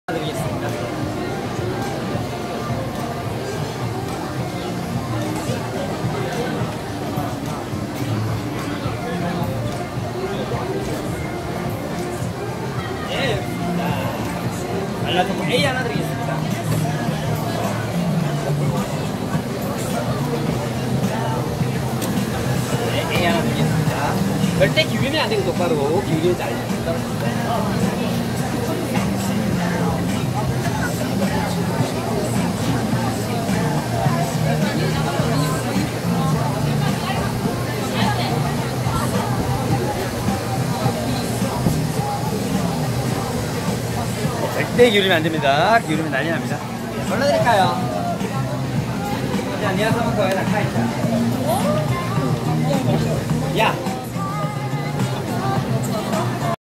32cm size amazing ice cream / 32cm 아이스크림 / Korean street food / 홍대 길거리음식
32cm size ice cream cone in Seoul

Location: 39, Jandari-ro 6-gil, Mapo-gu, Seoul, Republic of Korea